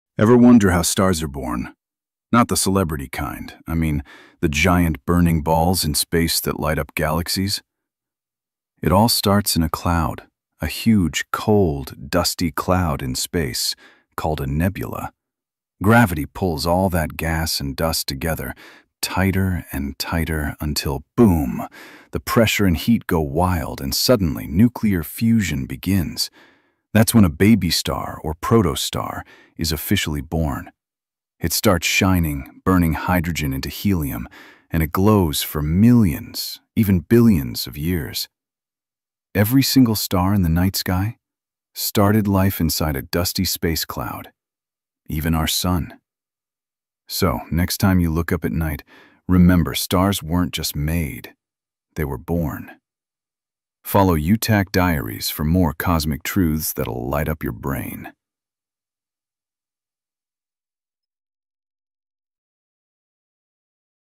How Stars Are Born 🌠✨ | Space Explained Fast
Ever wondered how a star begins its life? From clouds of dust to blazing fusion machines — it all starts in a nebula. 💫